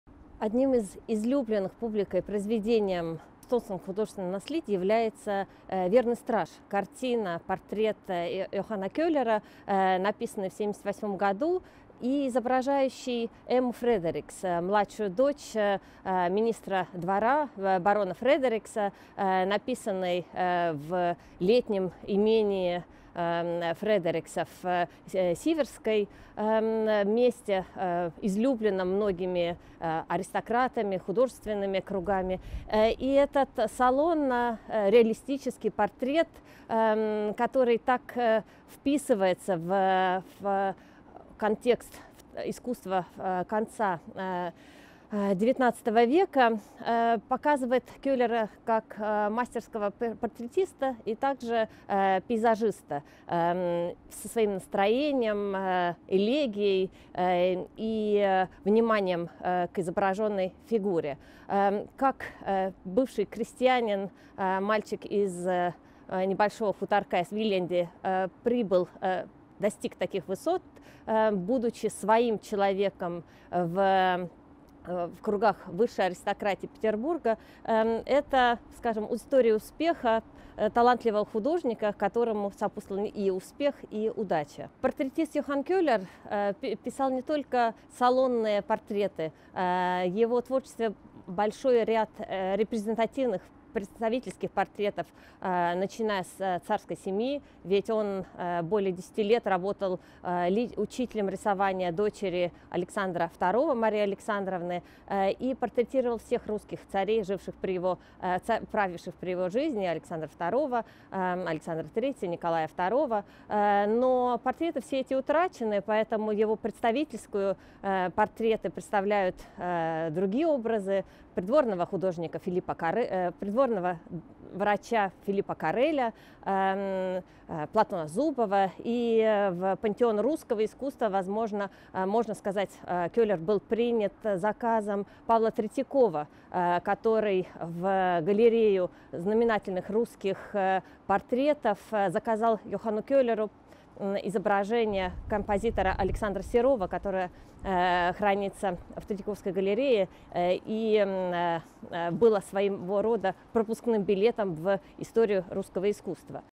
Johann Köler ja Peterburi kõrgseltskond. Aleksandra Murre
Esimene eestlasest kunstnik Johann Köler maalis 1878. aastal siinse kunstiloo ühe armastatuima portree „Truu valvur“. Viljandist pärit talupojast sai Vene keisririigi pealinna Peterburi kõrgseltskonna hulgas väga hinnatud kunstnik. Oma elu jooksul maalis ta portreesid kõrgete ametnike perekondadest ja ka kõigist oma kaasaegsetest keisritest.
Video on vene keeles, eestikeelsete subtiitritega.